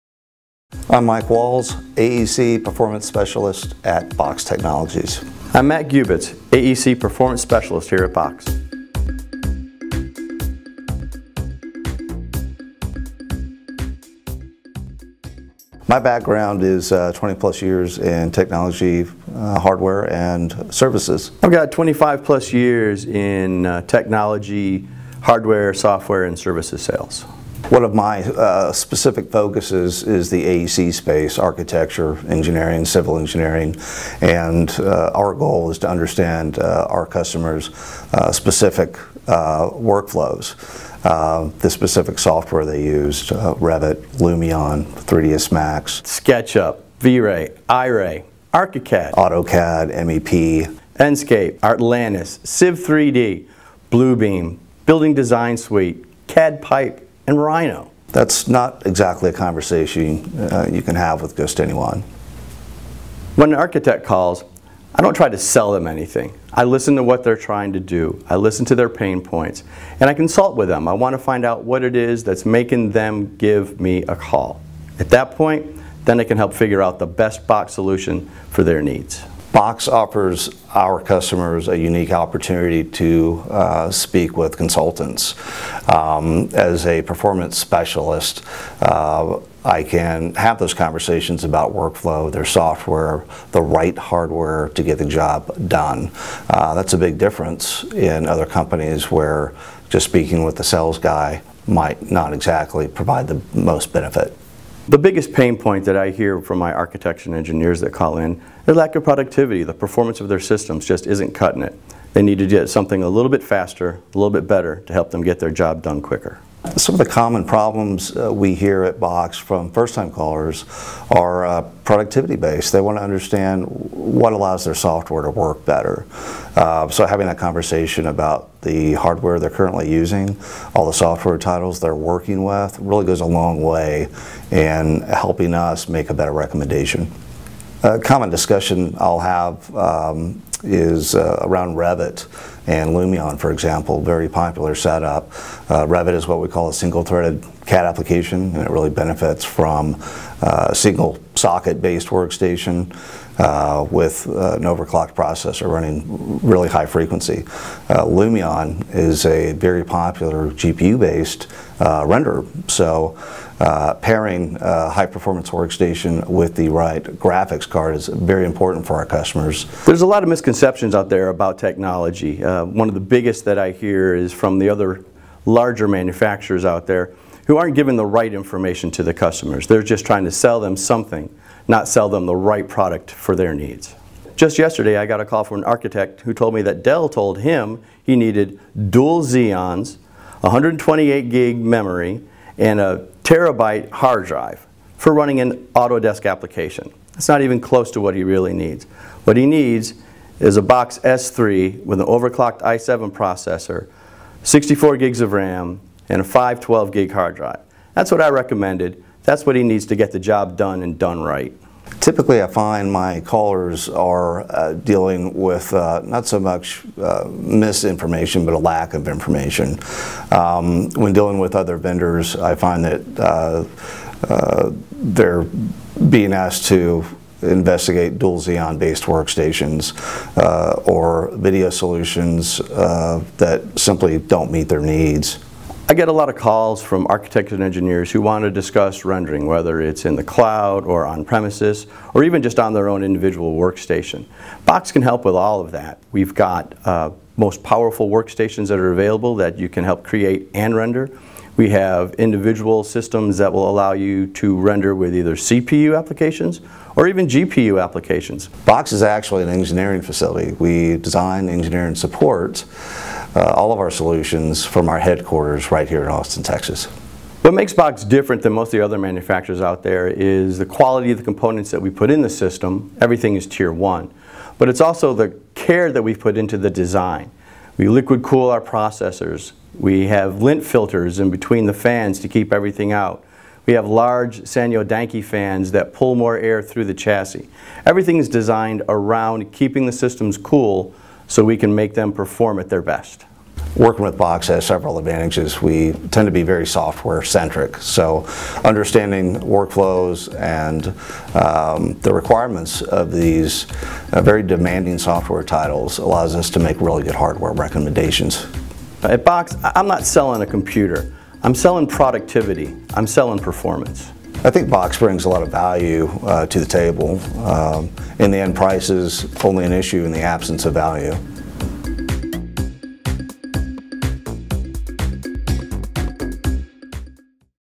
BOXX for AEC
AEC performance specialists Mike Walls and Matt Gubitz discuss architecture, engineering, and construction workflows, as well as optimizing your hardware, rendering, and the misinformation surrounding AEC workstation configurations.